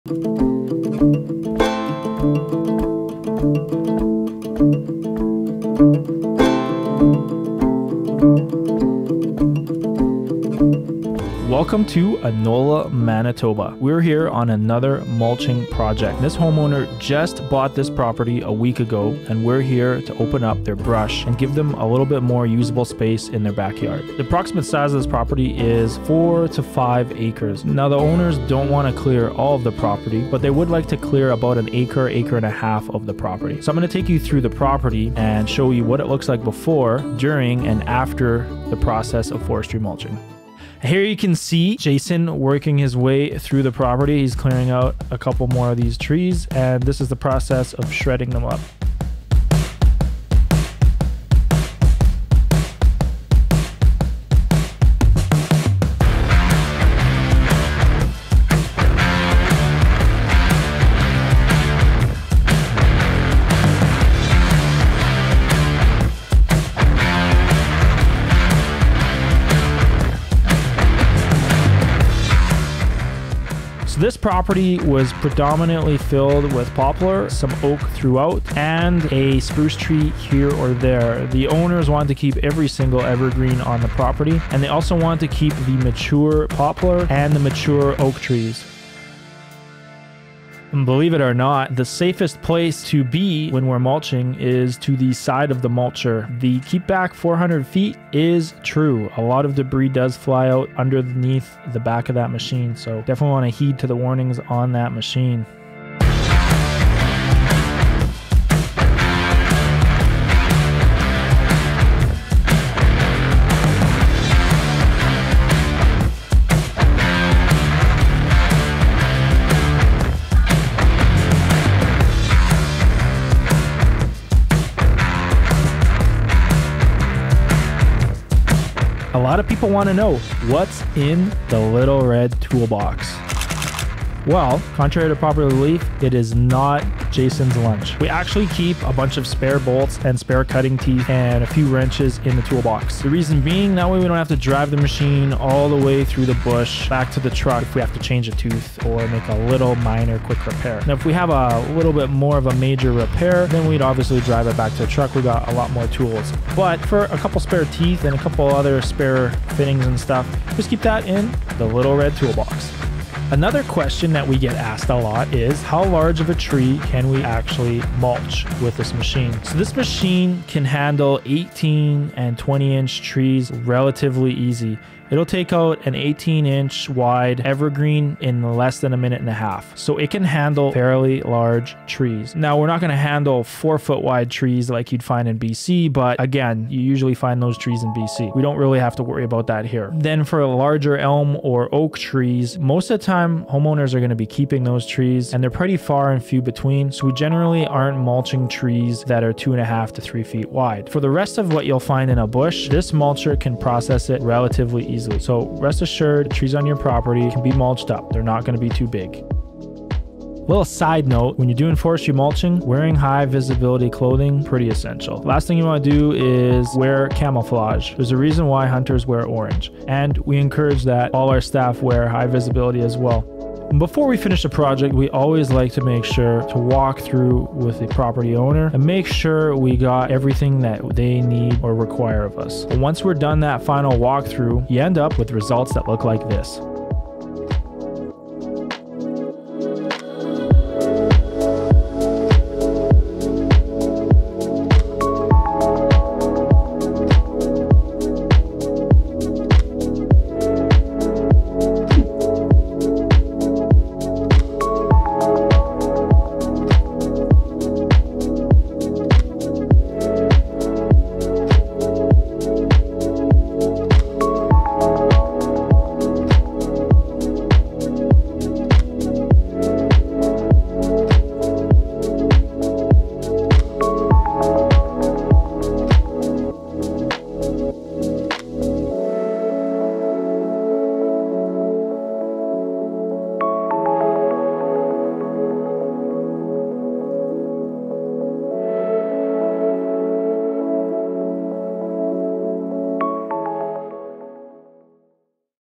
Brush Clearing In Rural Manitoba, Canada - Forestry Mulching
We head out of the city to take on a job in Anola MB. Our client has requested an acre of forest be cleared to extend their backyard space.

0:00 - Intro
0:41 - Initial Clear
1:30 - Safest Place to Be
2:09 - Our Little Secret (:
2:43 - How Big of a Tree Can This Mulcher Handle?
3:43 - Outro
3:53 - Final Results

OR

 property services